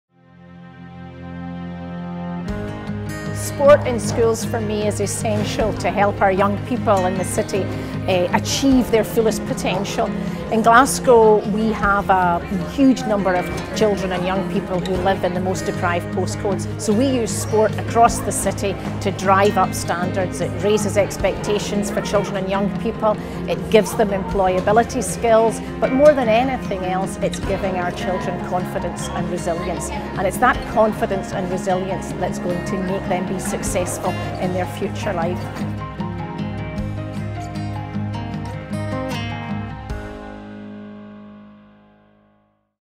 Maureen McKenna – Raising attainment through sport
The sportscotland School Sport Award is a national initiative designed to encourage schools to continuously improve the quality and quantity of physical education (PE) and school sport opportunities.